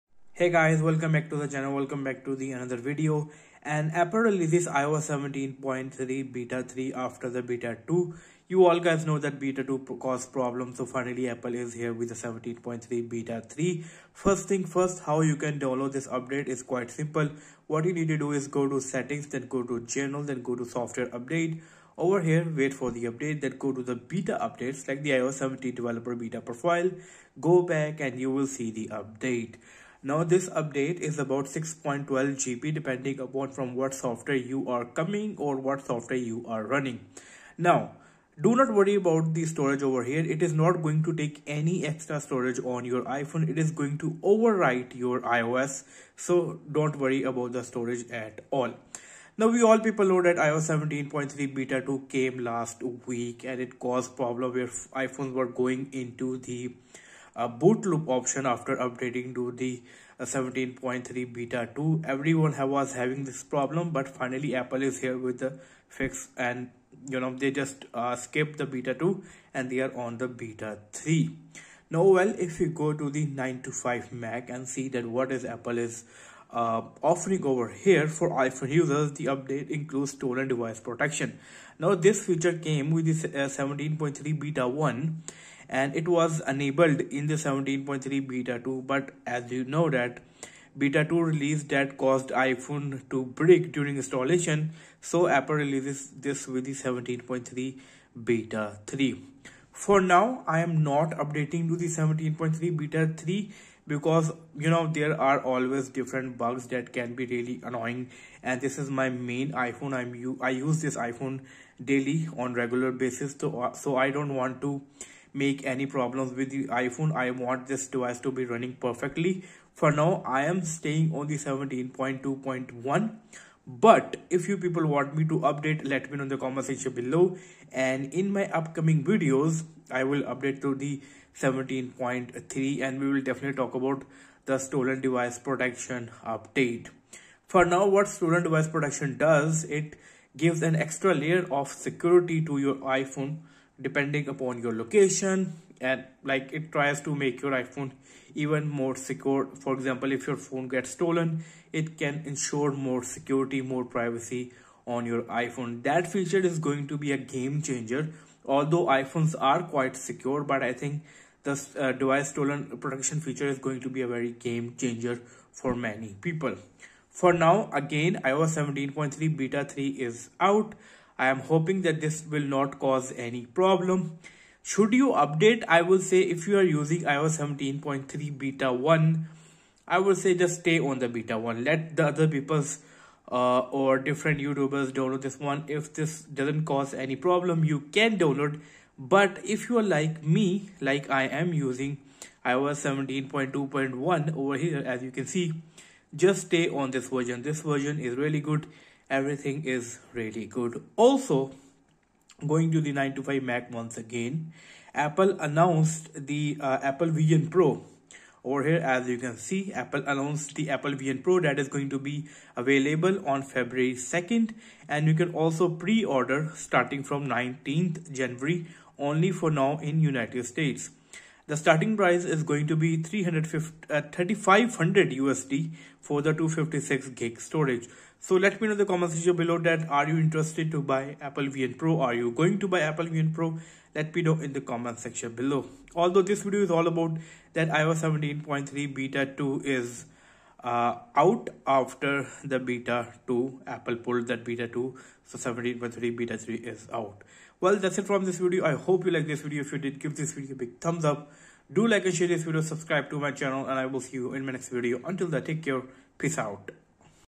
iOS 17.3 Beta 3 is Here - Should You Download?
Hey, Guys, I am your iOS Guy who is here to share his experience with iOS and iPhone.

Apple Releases iOS 17.3 Beta 3 Update
Last Week, iOS 17.3 Beta 2 was released but that was causing problems for iPhone so Apple pulled that update.
Now, iOS 17.3 Beta 3 is Here and available for download.
iOS 17.3 Beta 3 includes a Device Protection Feature.